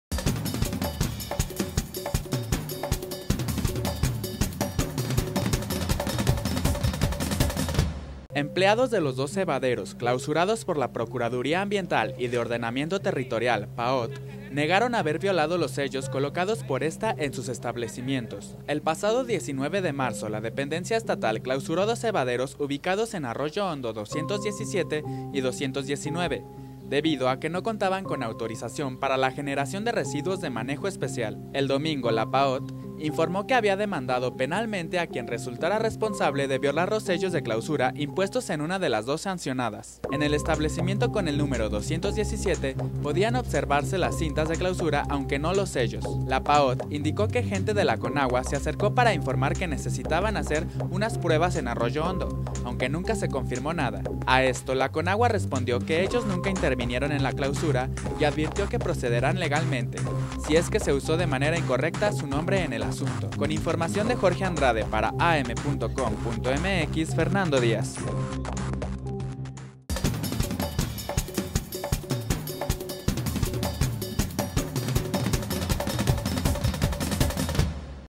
Niegan sebaderos acusaciones
Empresarios y empleados de los dos sebaderos clausurados por la Procuraduría Ambiental y de Ordenamiento Territorial (PAOT) negaron haber violado los sellos colocados por ésta en sus establecimientos.